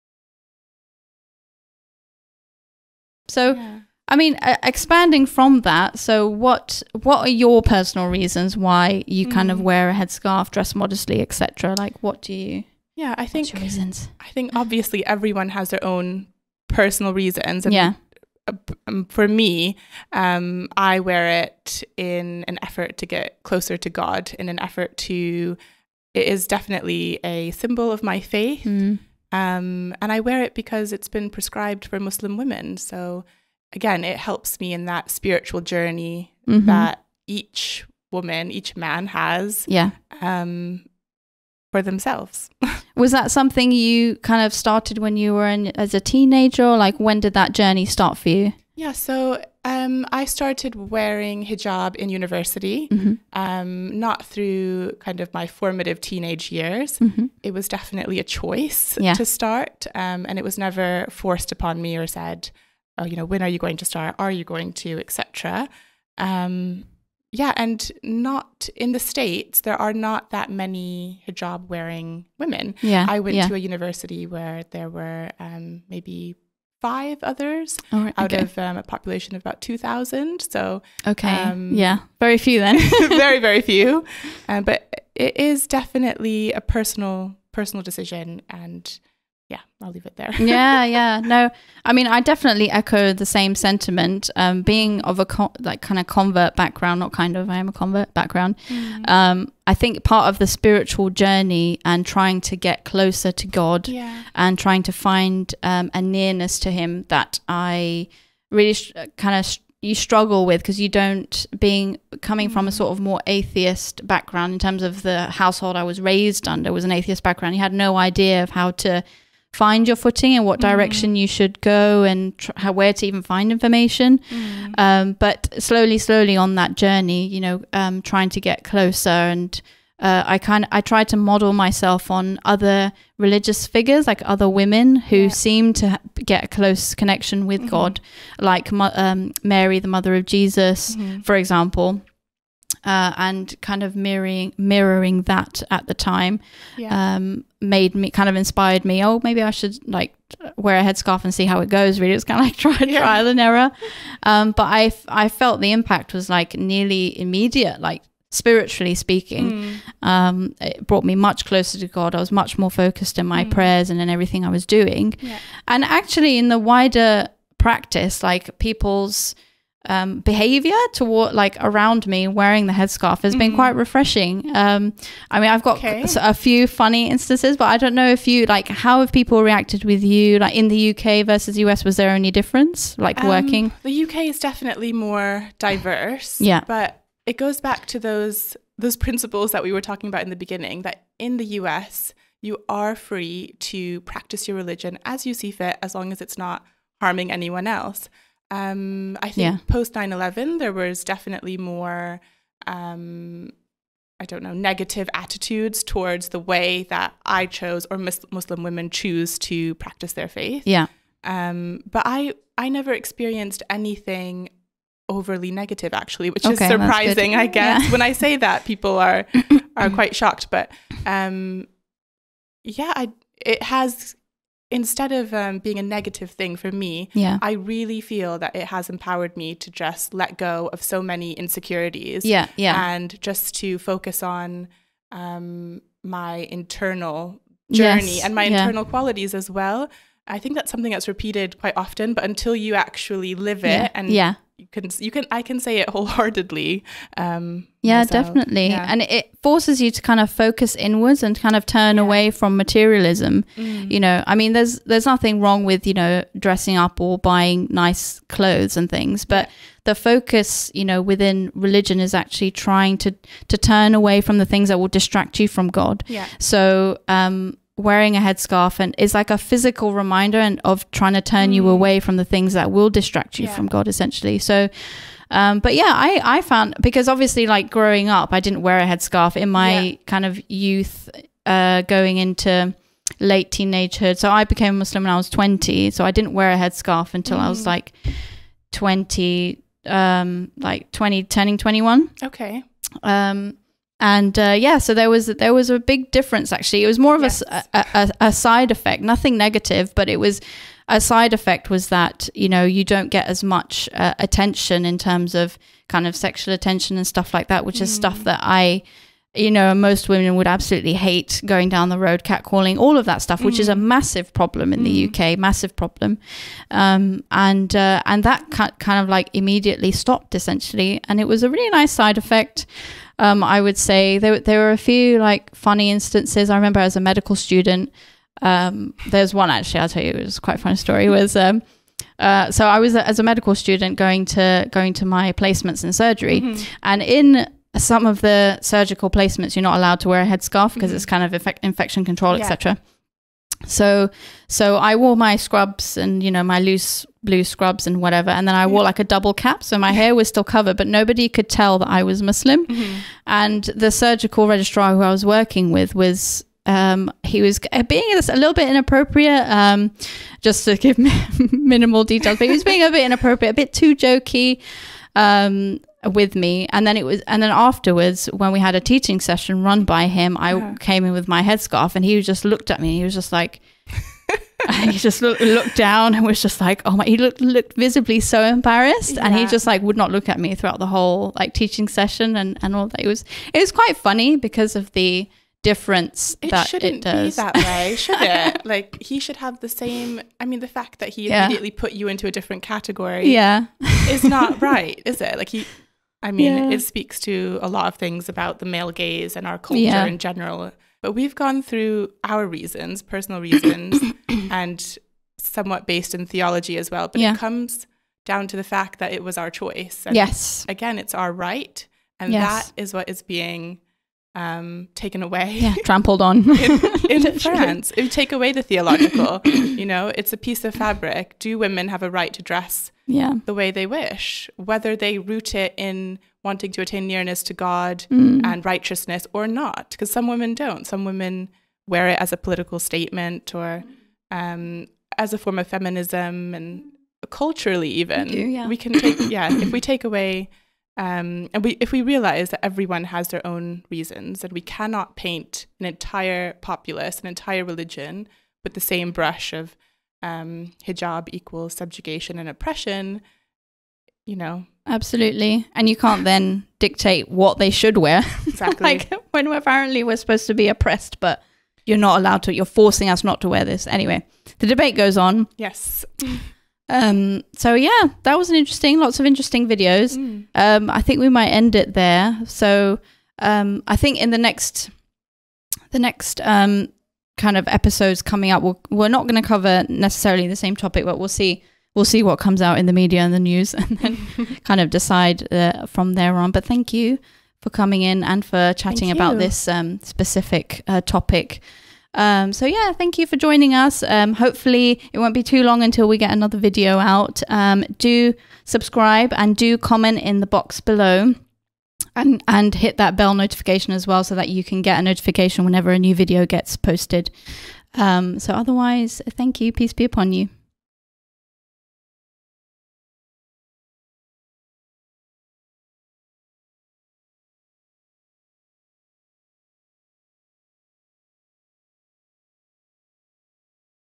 "The Doctor Was SHOCKED I Wear Hijab!"
Anne-Marie & Rizwana describe their experiences. What's been your experience?

Learn more about Islam Ahmadiyya, the peaceful religious philosophy behind Rational Religion, here: